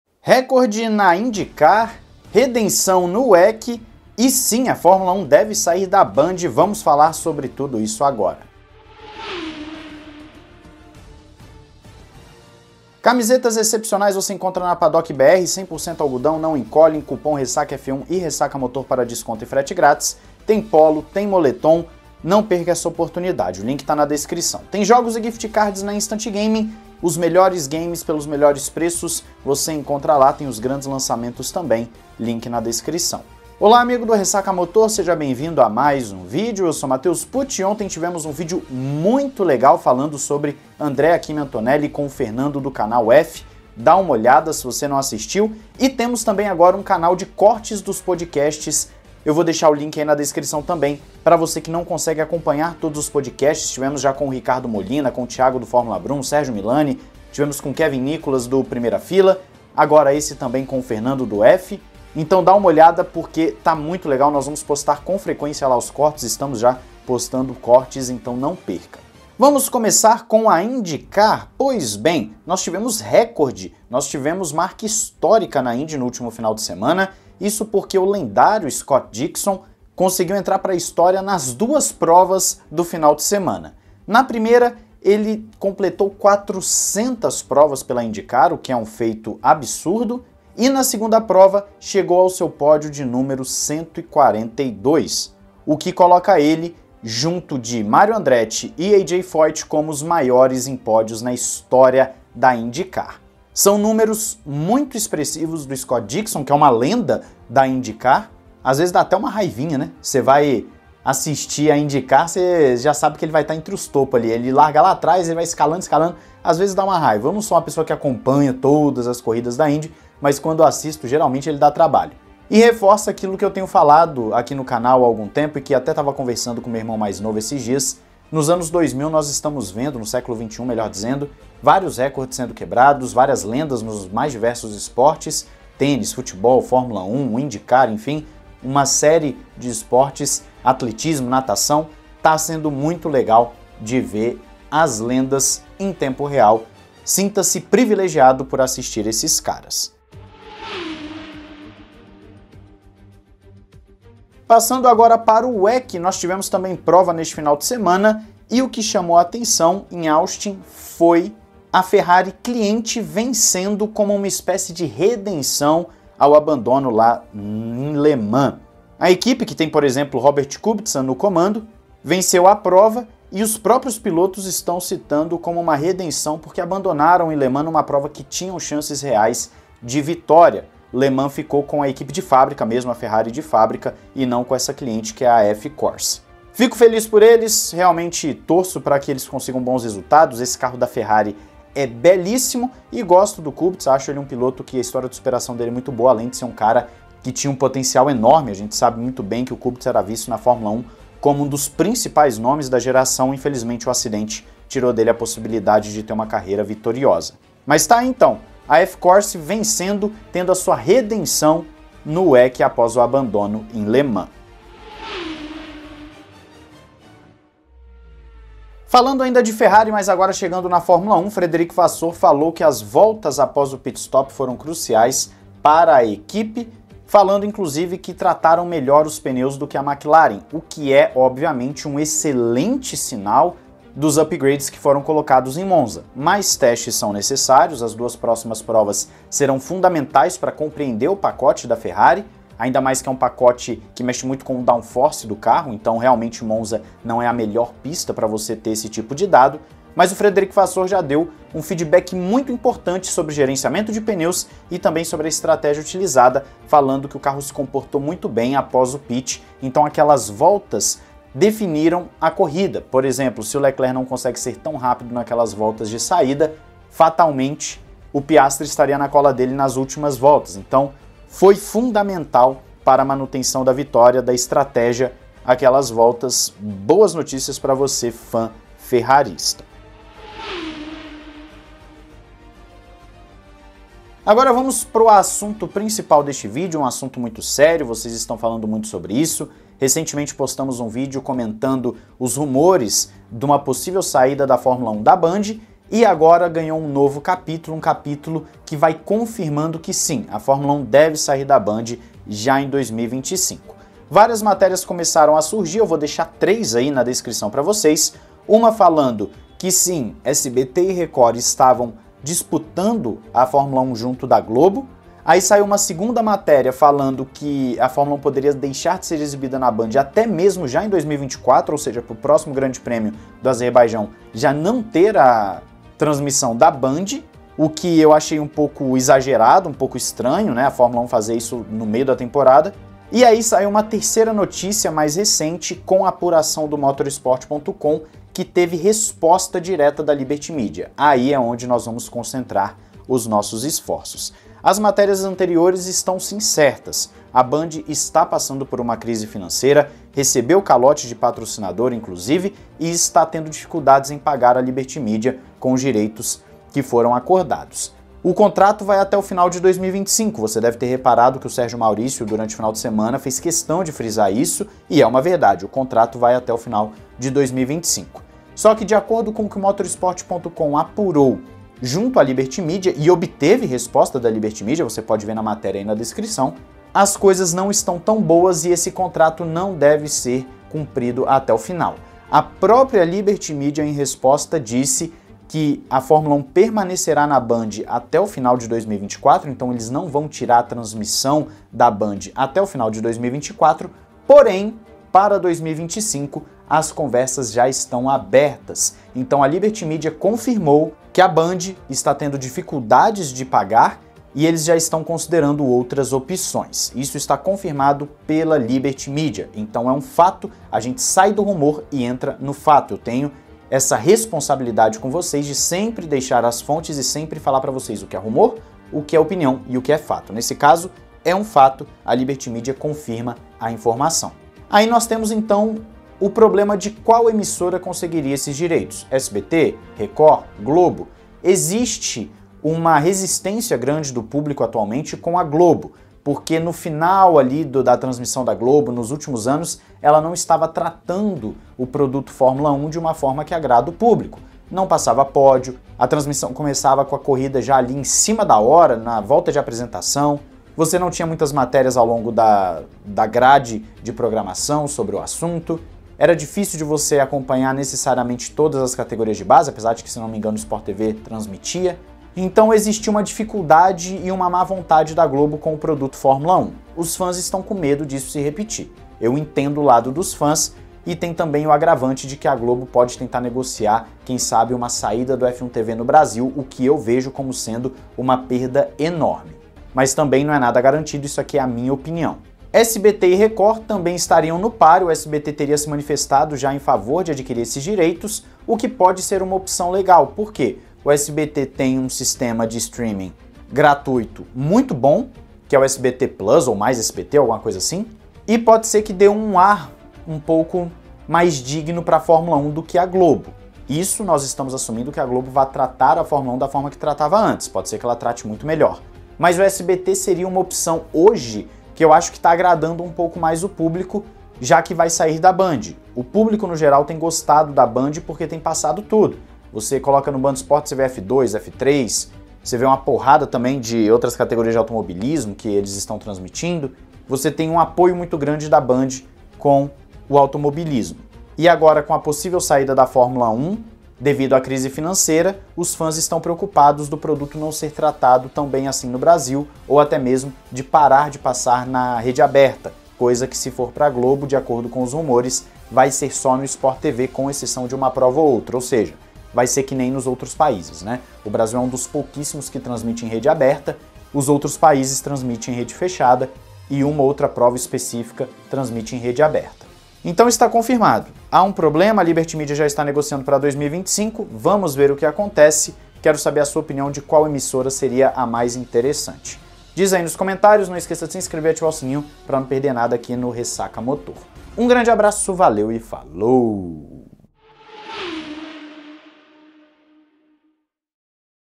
LIBERTY CONFIRMA NEGOCIAÇÃO DA F1 PARA 2025, REVELANDO CRISE NA BAND / INDY / WEC / FERRARI
Sergio Milani
@scuderiamilani (Twitter)

João 3:16

00:00 - Introdução
01:09 - Recorde na Indy
02:34 - Redenção no WEC
03:38 - Vasseur dá bom feedback do upgrade
04:45 - F1 vai sair da Band mesmo
06:22 - Liberty confirma negociação por 2025
07:07 - Qual emissora vai fechar negócio?